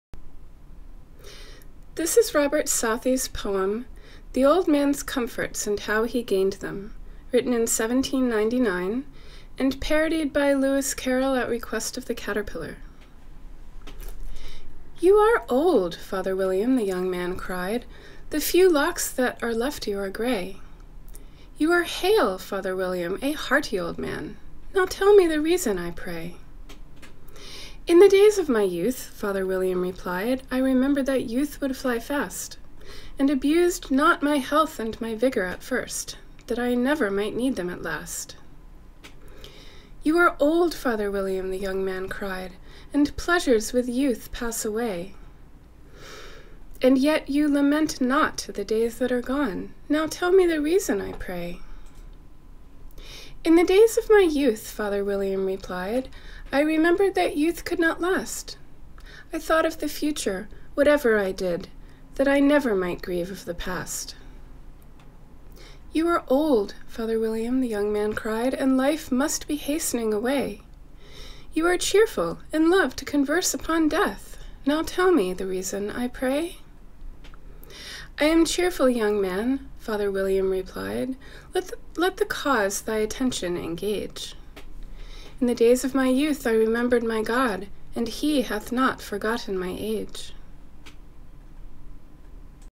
The Old Man's Complaints. And how he gained them, by Robert Southey, 1799.
The poem that inspired the Lewis Carroll parody, "You Are Old, Father William."
When I was a little kid, and read that book, I did not understand why the Caterpillar kept saying she had got it wrong, ha ha! If you ever wonder about any of the other parodies, puzzles, and other strange things about his books, I recommend "The Annotated Alice."